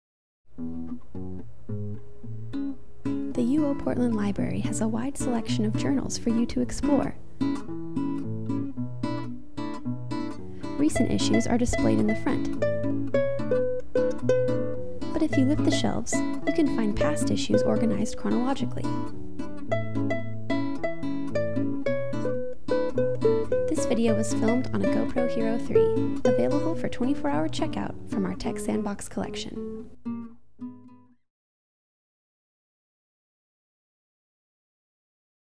UO PDX Library & Learning Commons: New Magazines
Do you know where you can find the most recent back issues of your favorite magazine?  Follow us and we'll show you!